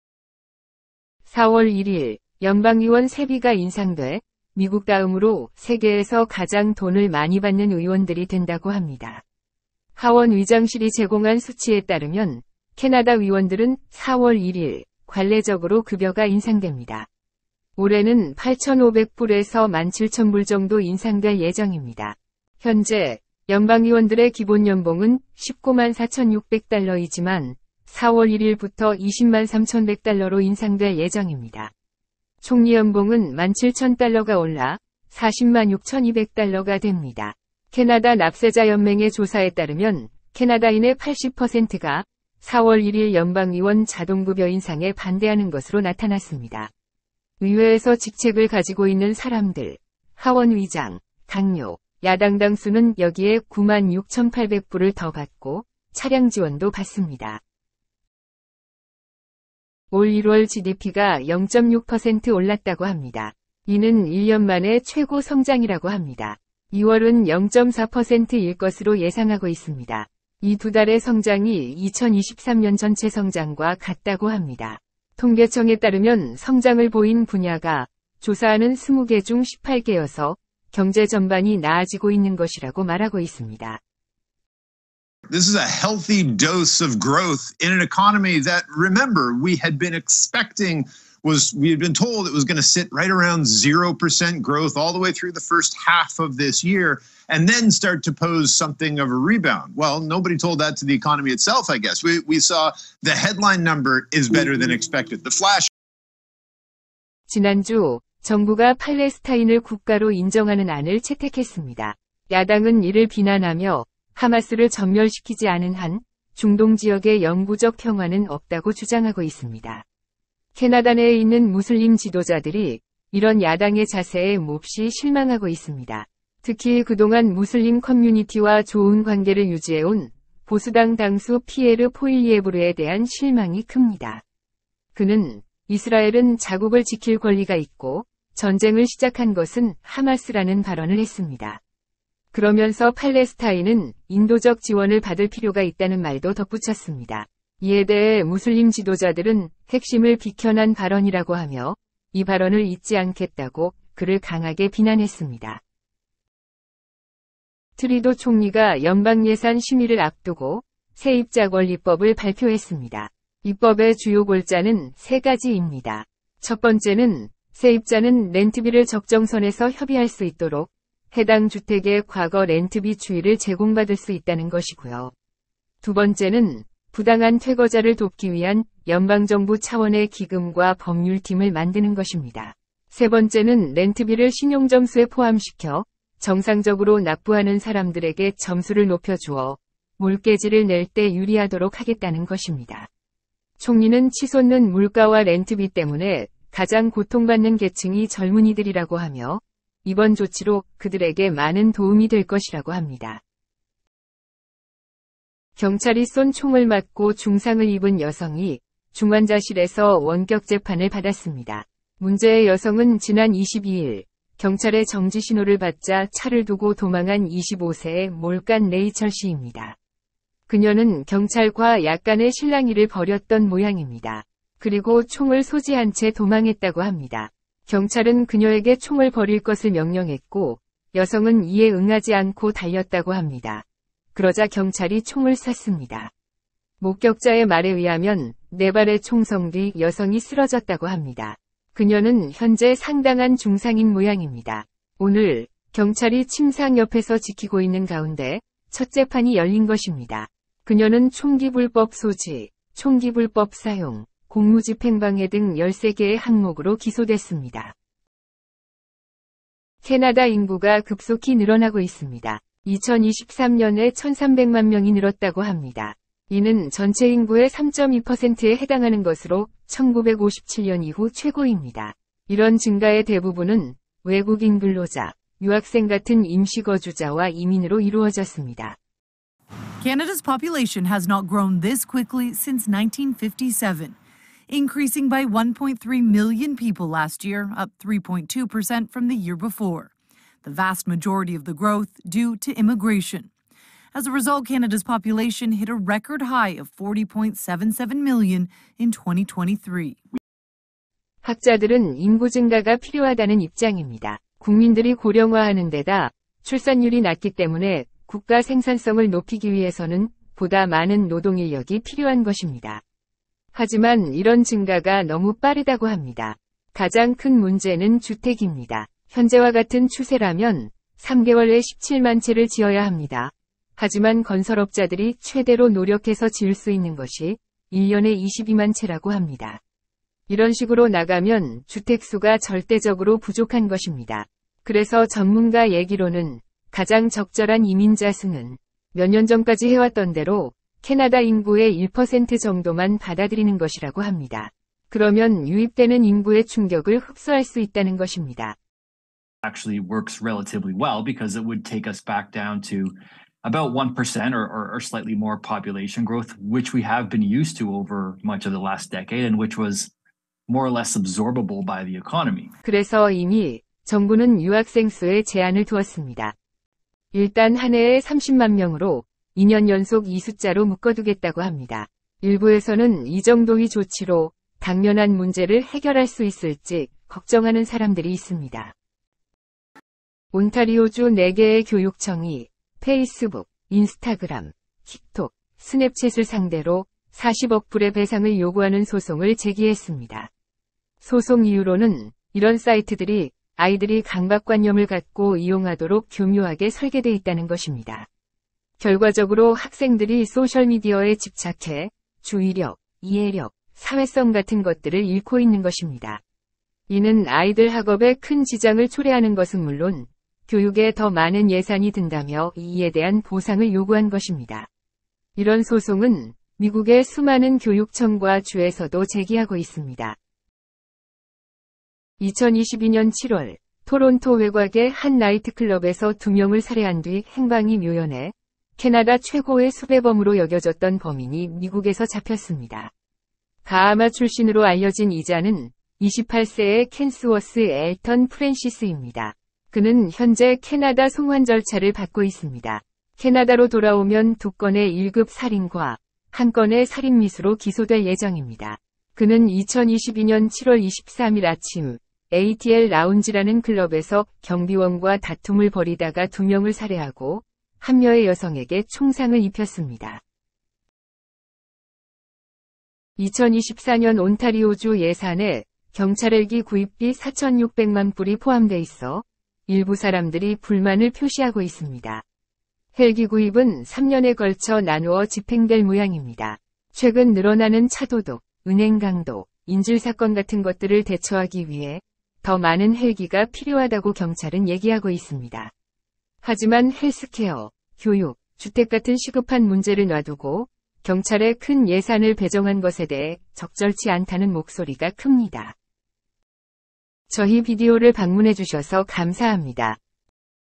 ✍️간추린 캐나다 뉴스(2024.3.28)
0:00 연방의원 세입인상
0:59 캐나다 경제 좋아진다
1:45 무슬림 야당당수 비난
2:37 새 세입자법
3:24 중환자실에서 재판받는 여성
4:19 인구증가 문제
6:19 교육청 소셜미디어 소송
7:01 최고수배범 체포
7:41 경찰헬기구입 반대